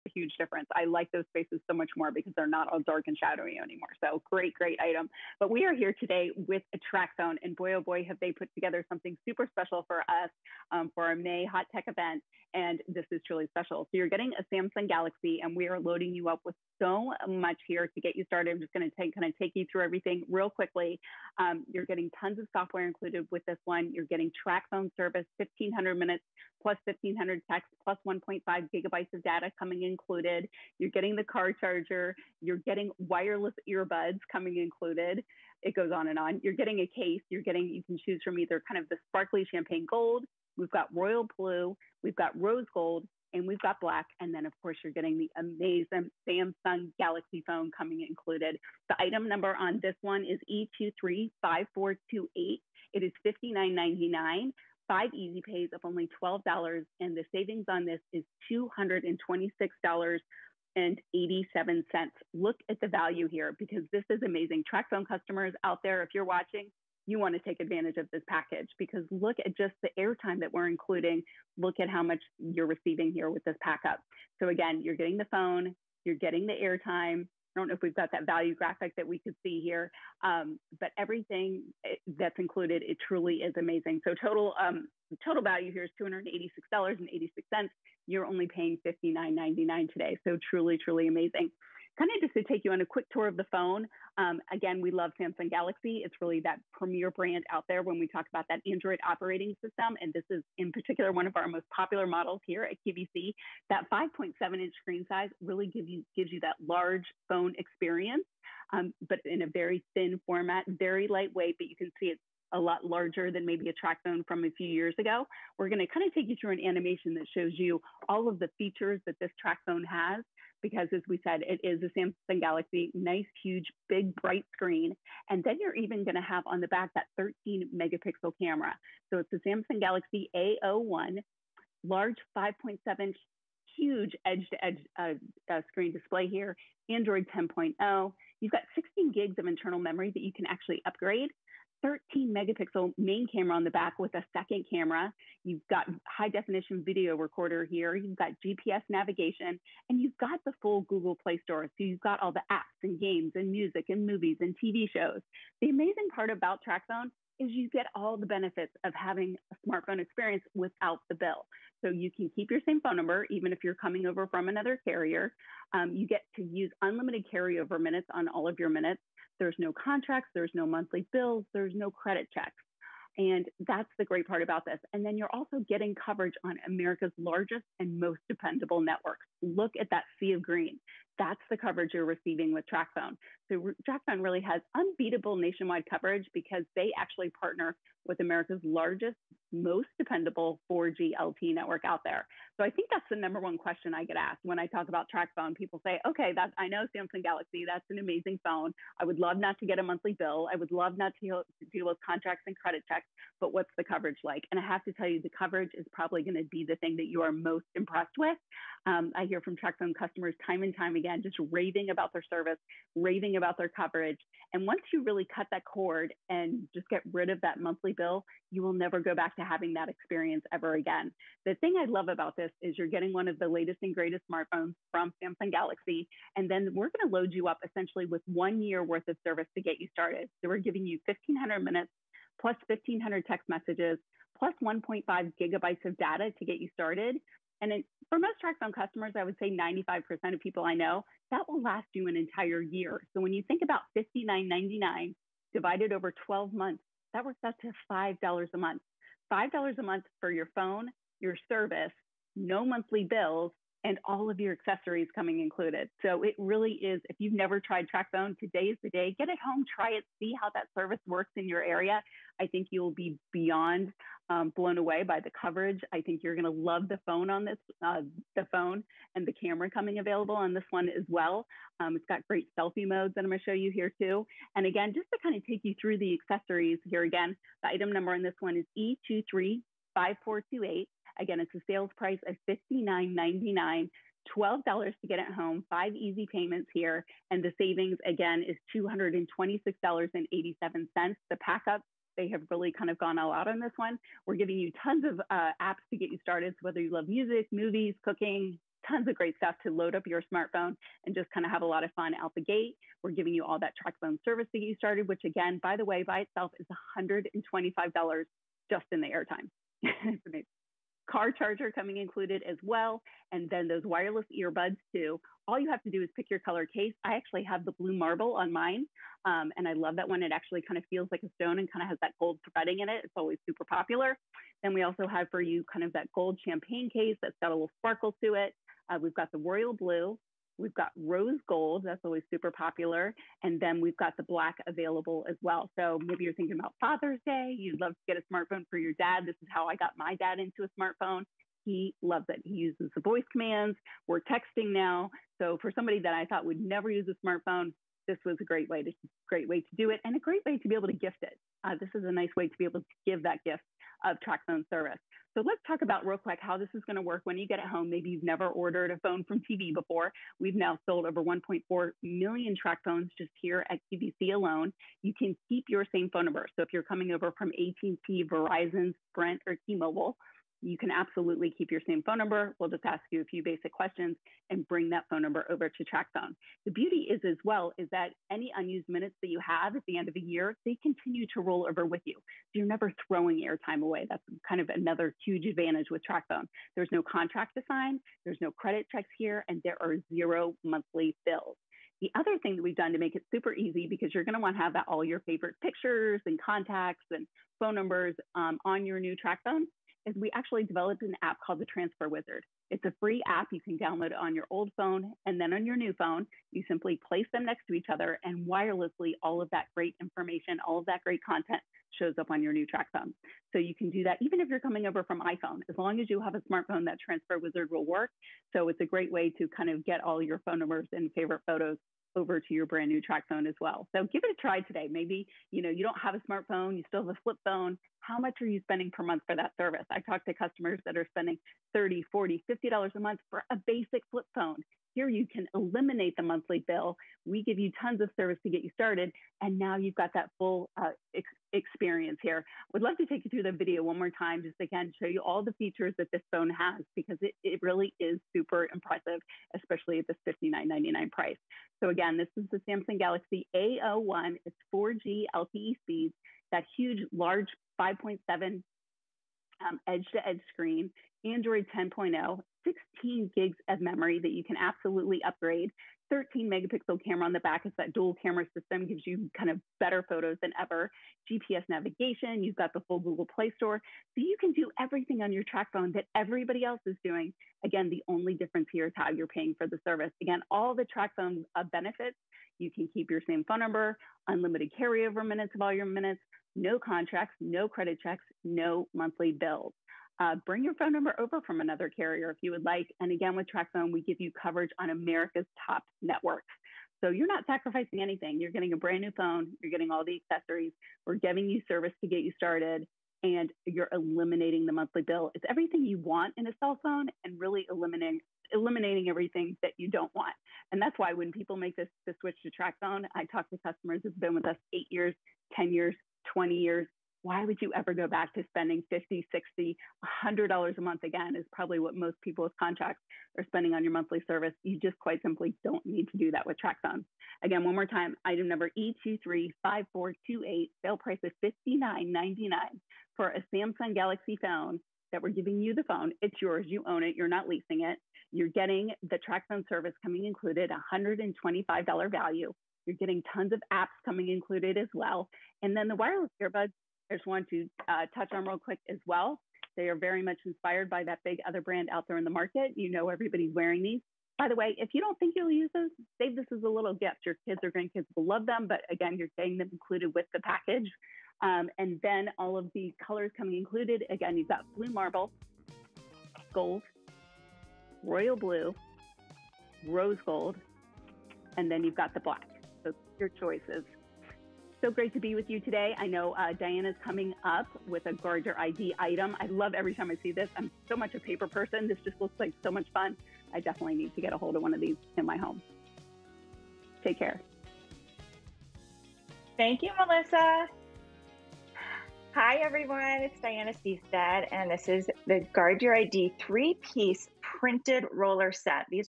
Samsung Galaxy A01 5.7" HD+ Tracfone w/1500 Min/Text/Data for 365 Days on QVC
Samsung Galaxy A01 5.7" HD+ Tracfone w/1500 Min/Text/Data for 365 Days

Scroll through your feeds, stream a whole series, game for hours, or do more all at once with this Samsung Galaxy A01 smartphone. Complete with an edge-to-edge 5.7&#34; Infinity V-display with bright and bold visuals, this easy-to-use option also captures sharp photos and shareable videos with a versatile dual rear camera combined with a sweet selfie front camera. And it's easier than ever to stay in touch with your family and friends with 1,500 minutes, 1,500 texts, and 1.5GB data. From Samsung.